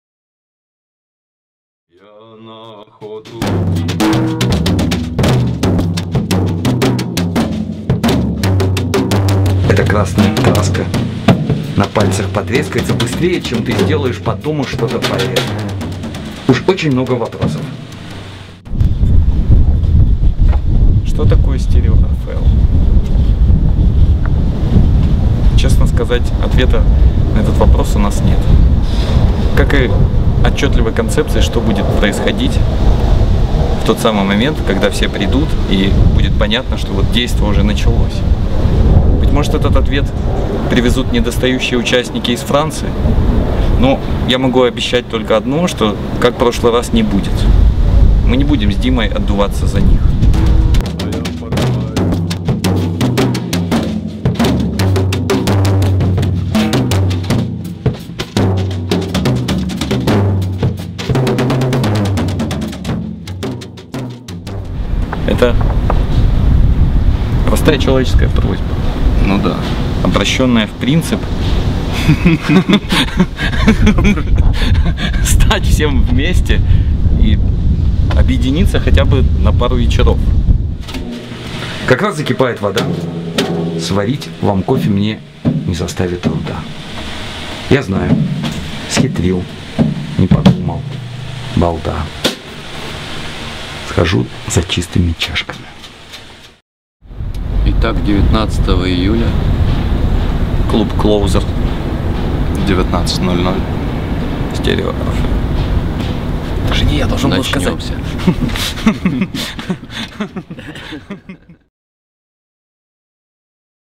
stereOrpheo. Концерт в Киеве. Анонс.
О мире, о любви, о природе различных столкновений расскажут со сцены участники StereOrpheo - экс-участник “Океана Ельзи” и “Esthetic Education” Юрий Хусточка, участник “Вагоновожатых” Антон Слепаков, экс-актер театра ДАХ Дмитрий Ярошенко и художник Никита Кравцов.

Презентация созданного участниками проекта StereOrpheo “манифеста о мире и любви” пройдет в формате их живого выступления, демонстрации созданного ими видео “После всех столкновений” и презентации книги “WAR IS OVER”, изданной S.T.ART в рамках проекта.

Организатор - S.T. ART
Куратор - Татьяна Швед-Безкоровайная
Менеджер - Ольга Иванова

Количество билетов ограничено. Приобрести их можно по ссылке

Аккредитация для СМИ:
Пресс-служба S.T. ART
Евгения Лиходовская